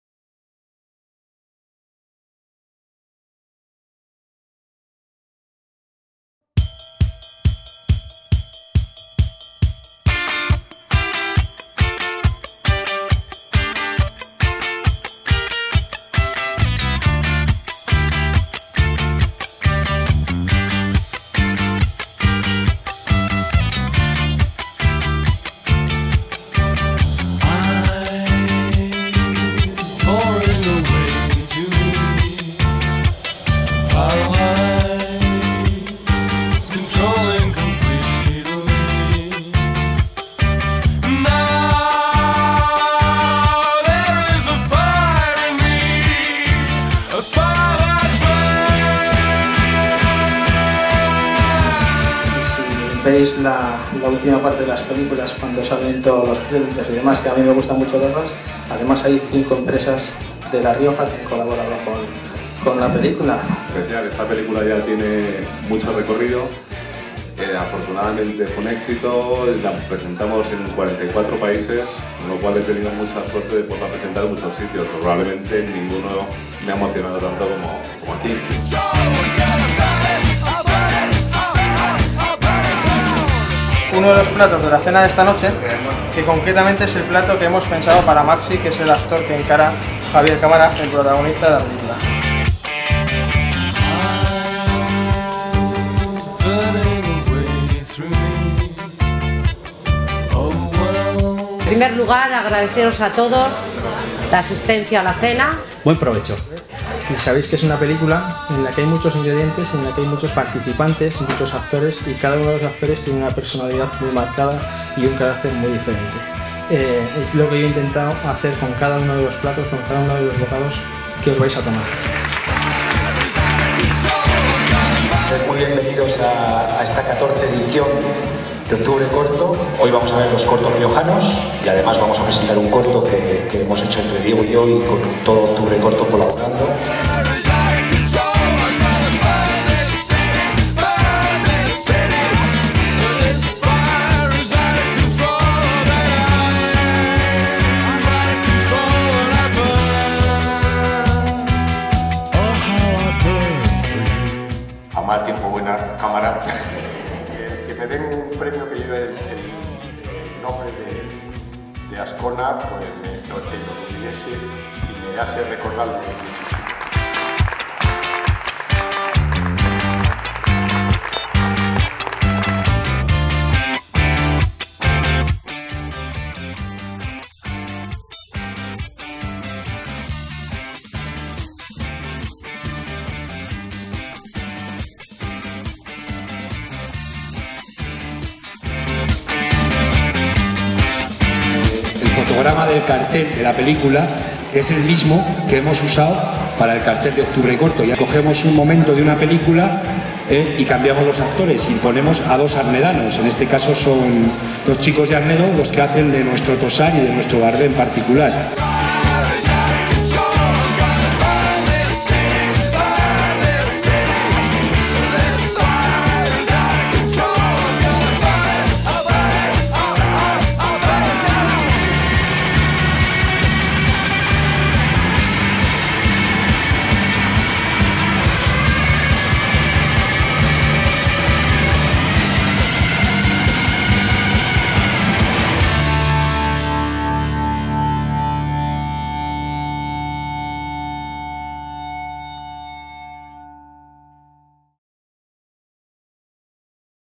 2012. Making Of Octubre Corto.
Octubre Corto, se ha consolidado en este tiempo como el principal evento cinematográfico de La Rioja y como una de las citas fundamentales en el panorama del cortometraje nacional. Desde el año 2015 los cortos ganadores, en la categorías de ficción, y documental, califican para los Premios Goya de la Academia de las Artes y las Ciencias Cinematográficas.

Video realizado durante la décimo cuarta edición del Octubre Corto 2012.
Grabado por:
Francisco Javier Sandín
Editado por:
David Fernández-Pellón Vallejo